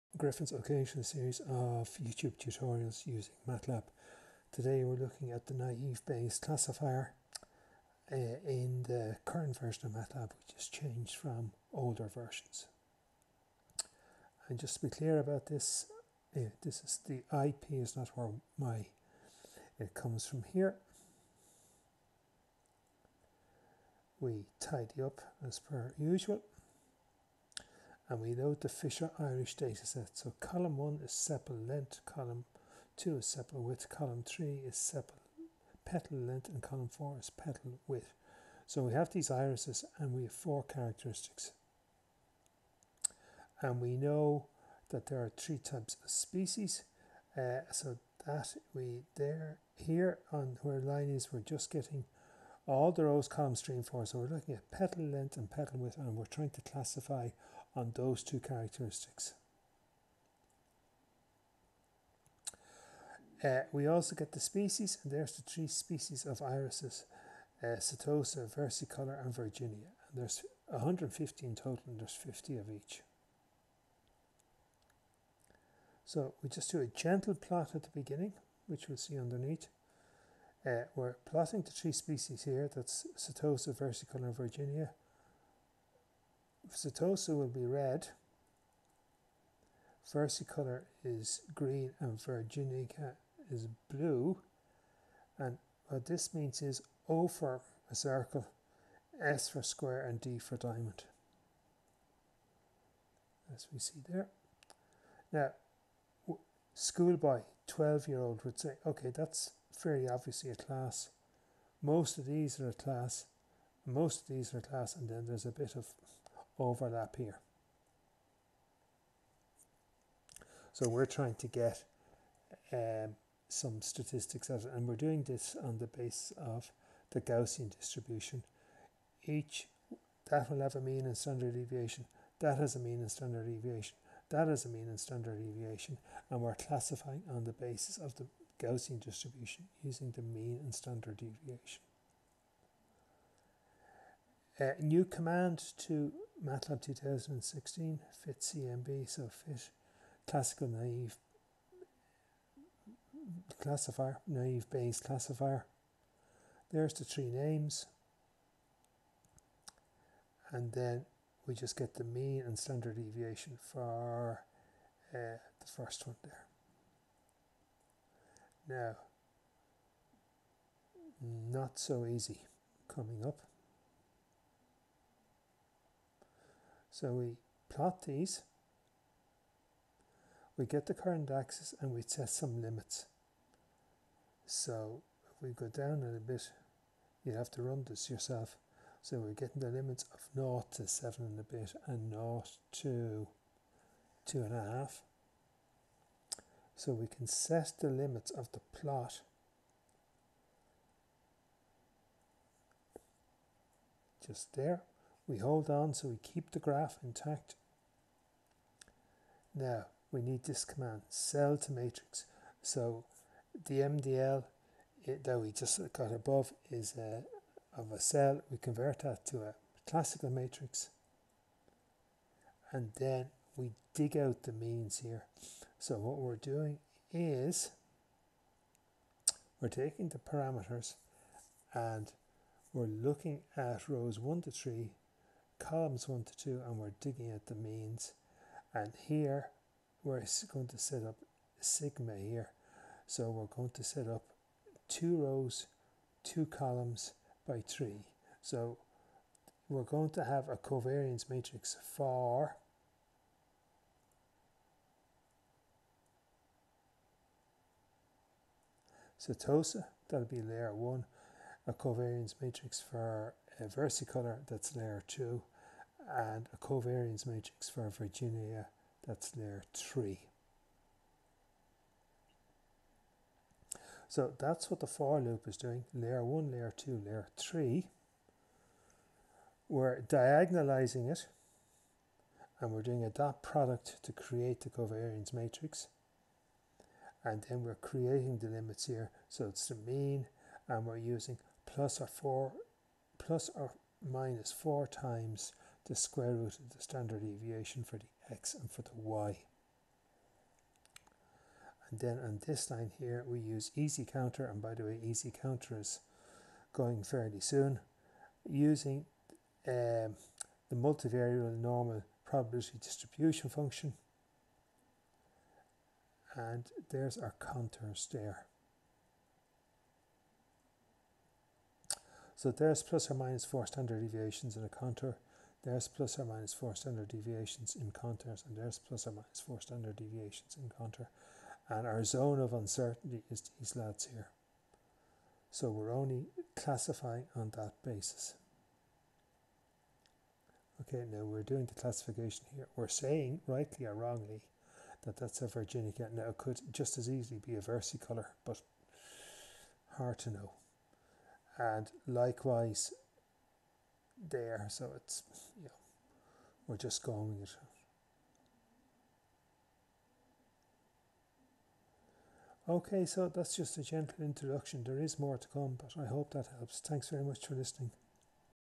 Naive Bayes Classifier in Matlab
This is  a short demo of how to  implement a naive Bayes classifier in Matlab